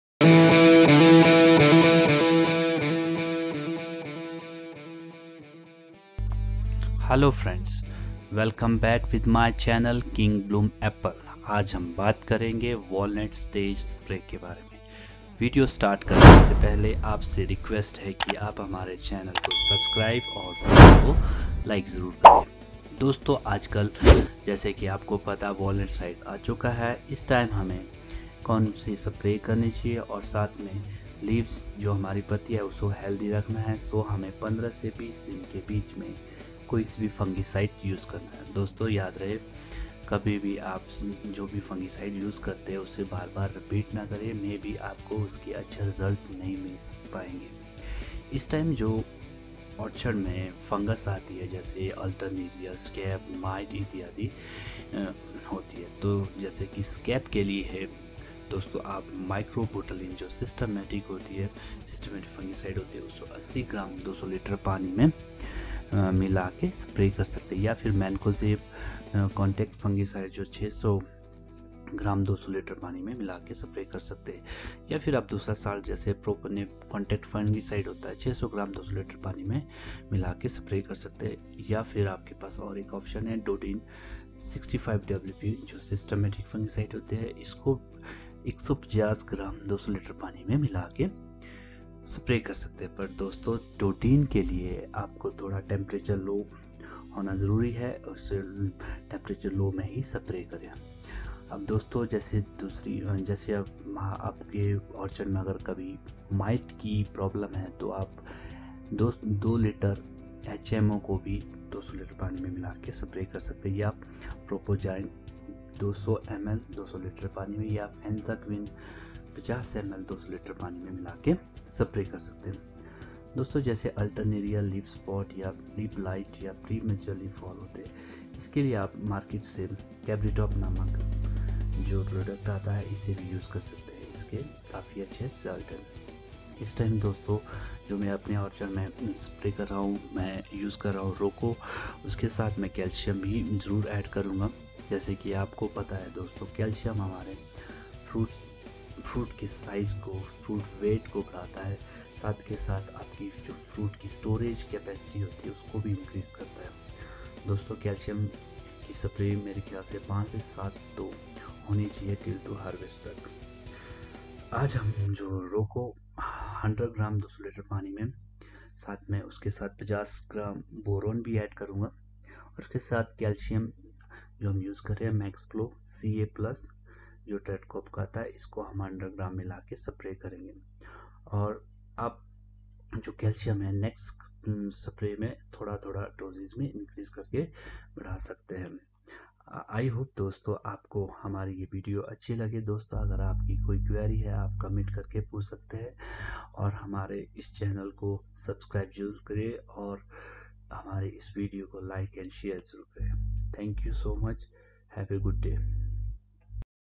Walnut Stage Spray || अखरोट आकार के सेब पर स्प्रे || King Bloom Apple
Walnut stage spray is very important. Fungicides spray is necessary to keep leaves healthy, it protects leaves from various fungal diseases.